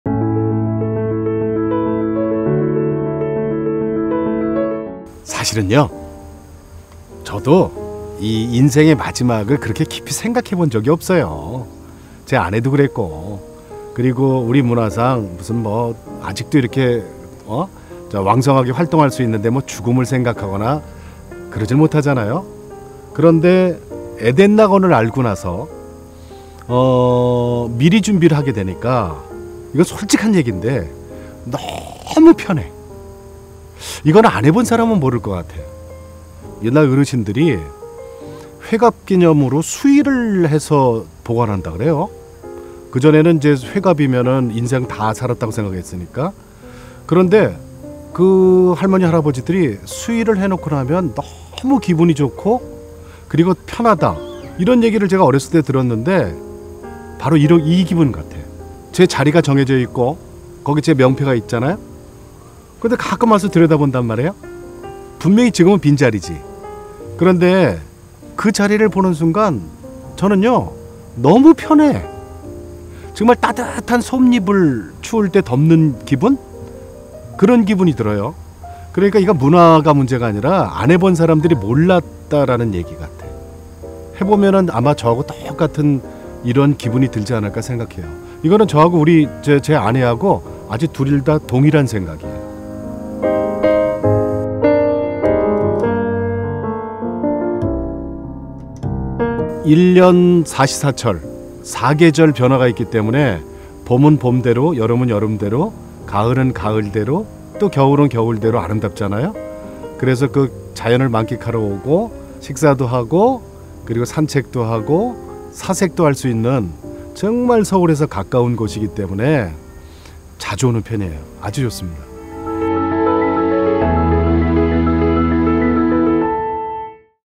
[에덴 피플 스토리] 최선규 아나운서
"미리 구입하니 이렇게 편안합니다" - 최선규 아나운서

에덴낙원을 선택한 많은 오피니언 리더들은 봉안당을 미리 준비하고 에덴낙원 메모리얼리조트에서 평생 회원권으로 가족들과 행복한 추억을 만들어가고 있습니다. 날마다 더해지는 좋은 사람들의 행복한 이야기를 여기 소개합니다.

인생과 마무리에 대한 에덴낙원 멤버들의 생각을 전해 드립니다.